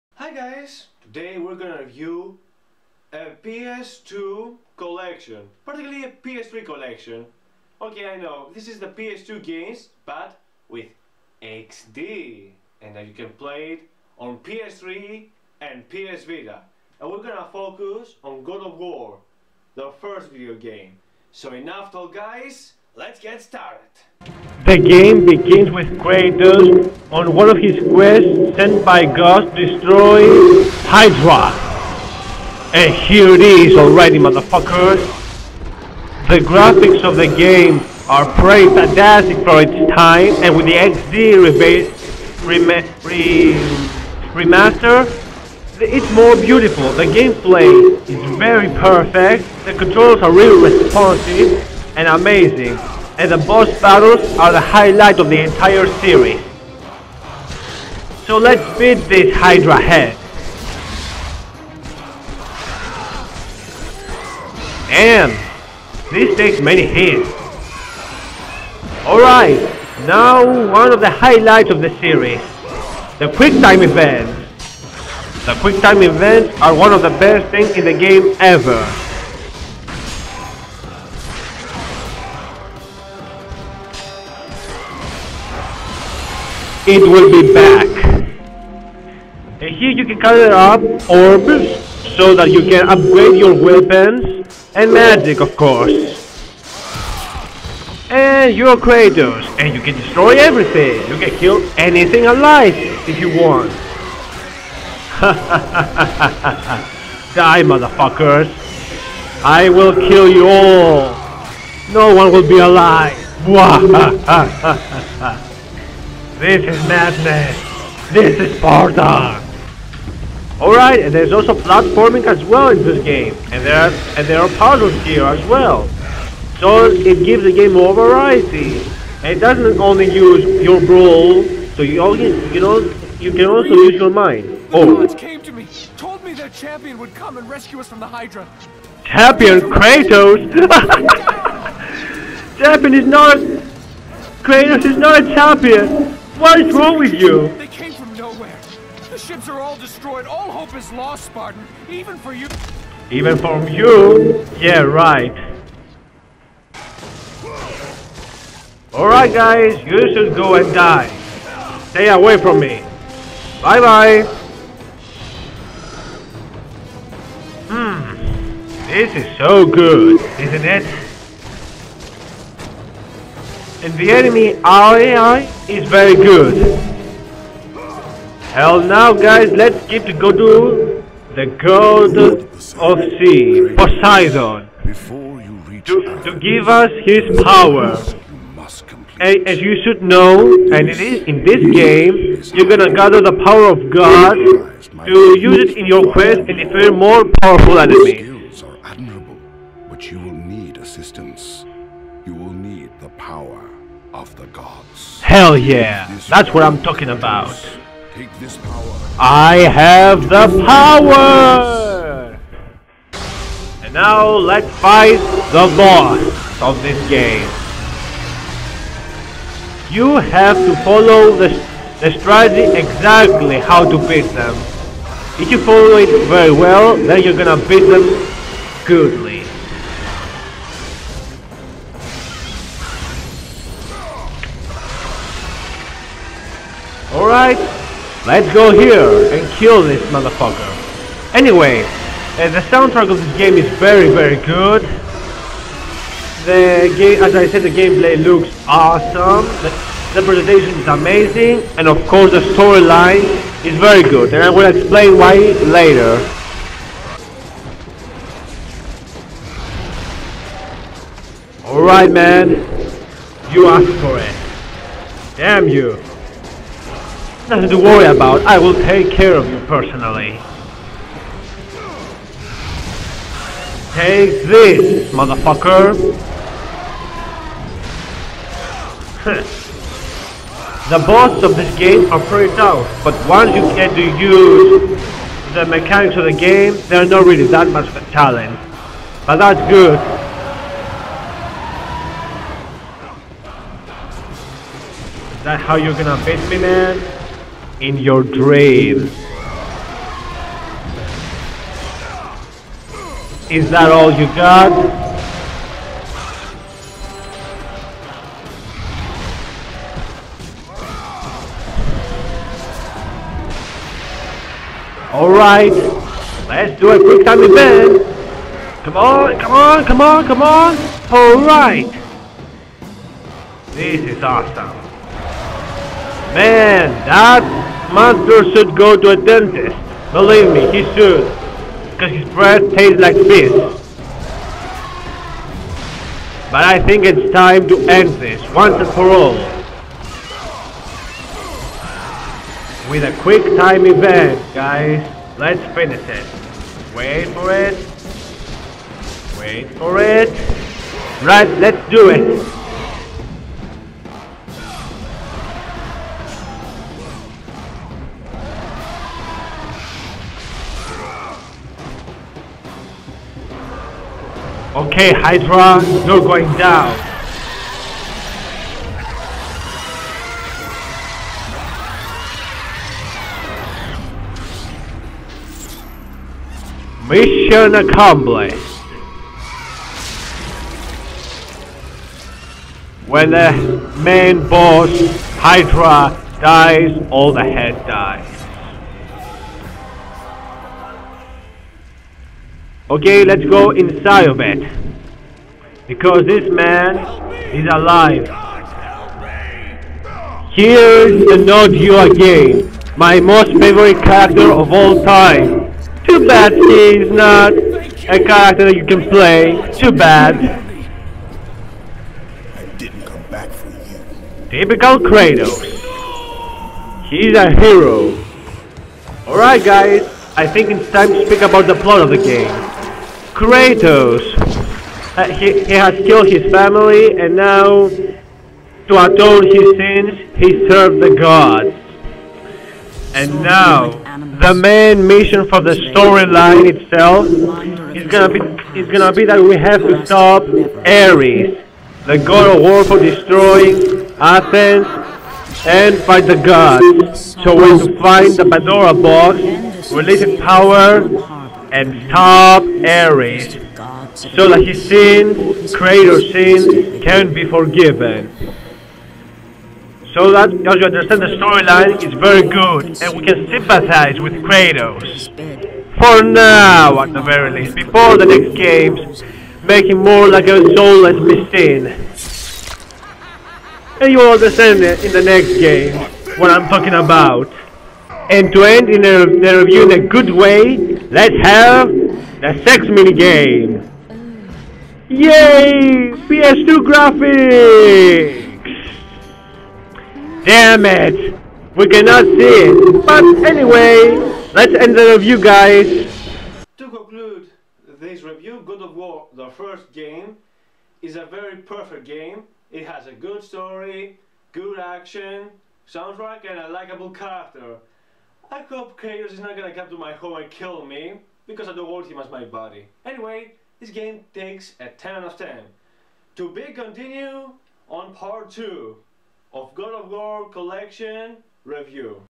God of War Collection part 1 Review
Sonicfan is going to review God of war collection for the PS3 the first part